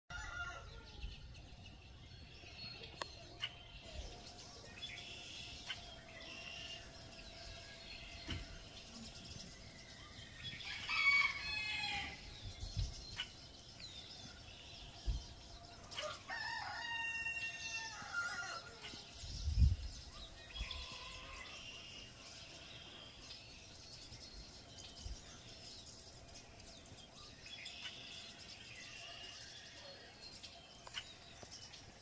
Cool Clouds & Gorgeous Skyline (Birds singing theirs hearts out!)
Yesterday was such a beautiful spring day in the North. The sun was shining, the rain clouds were cool-looking & far away, our roosters were crowing, our hens were clucking, the songbirds were singing, and the animals were happy.